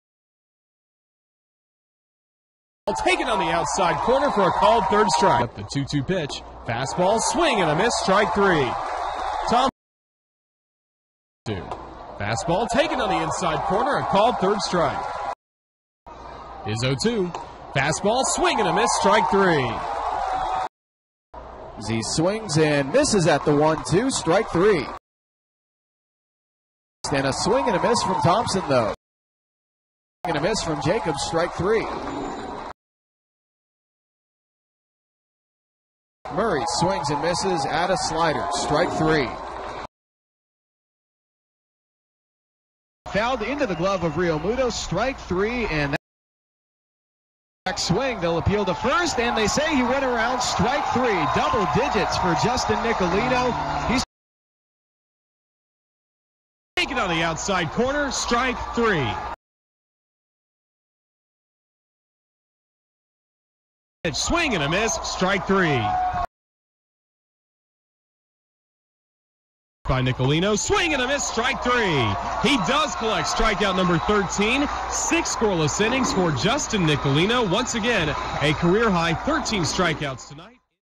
Jacksonville Suns starter Justin Nicolino fans 13
Watch Jacksonville Suns pitcher Justin Nicolino strike out 13 against the Birmingham Barons on Aug. 16, 2013